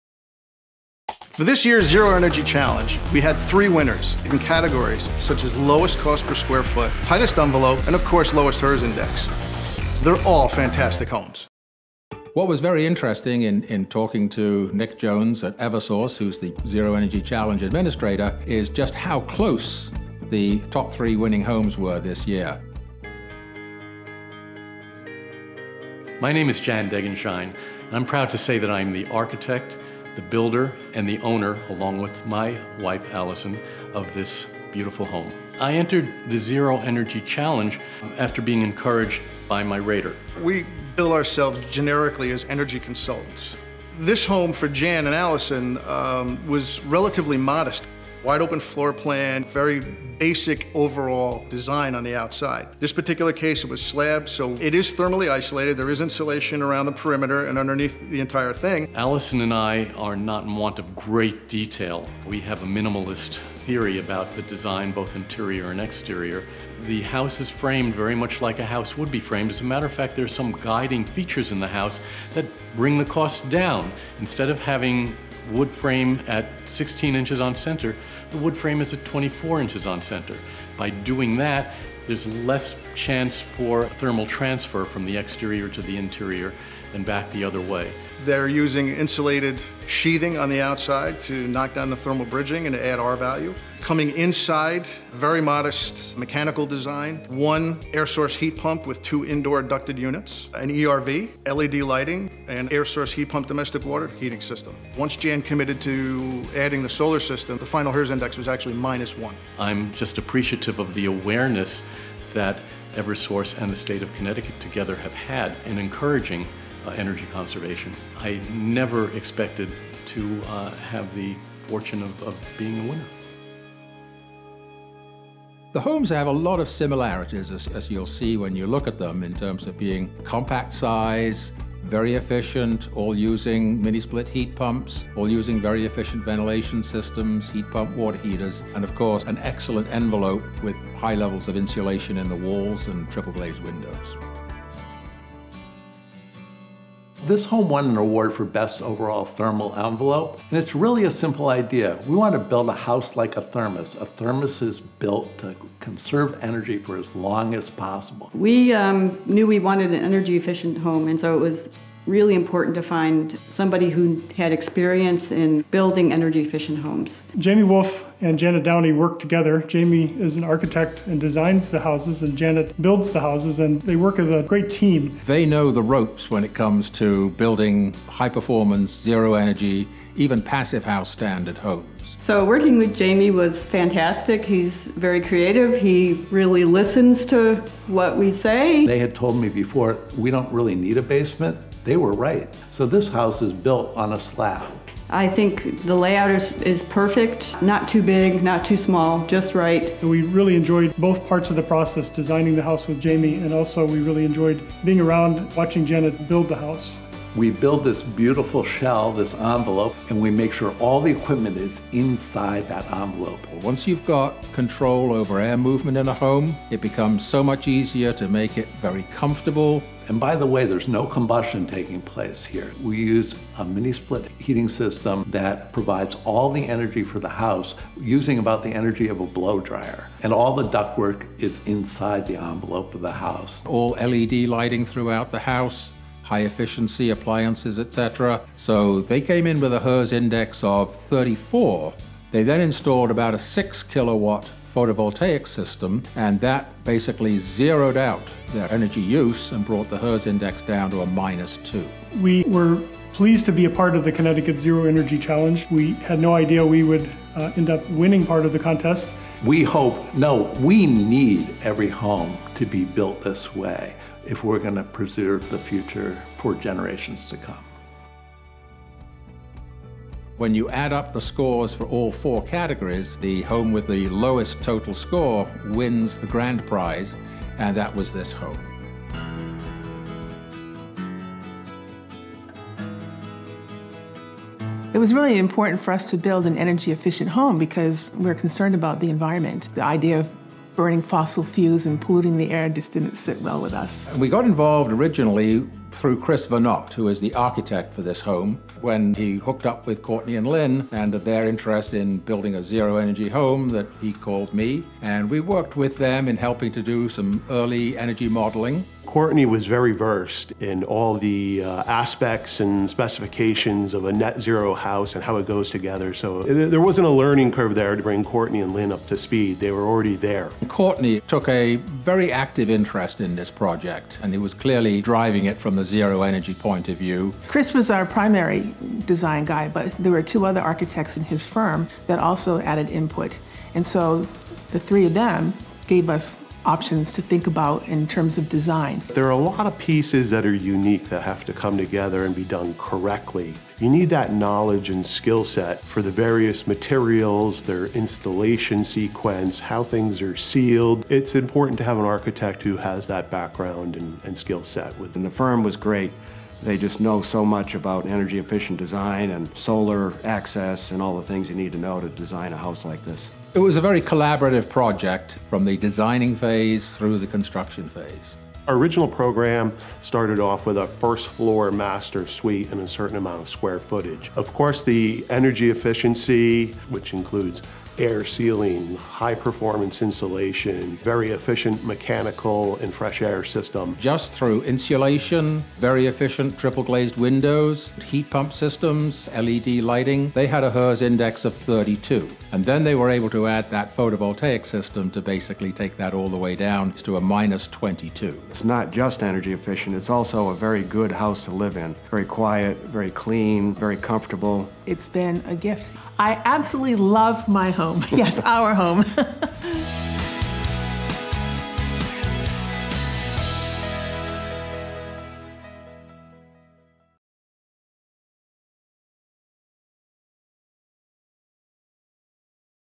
2017 Zero Energy Challenge Winners
Did you think a net zero energy home was a think of the future? The winners of the 8th annual Energize CT Zero Energy Challenge beg to differ. See why and how these home owners, builders and architects joined the challenge and what they learned in the process.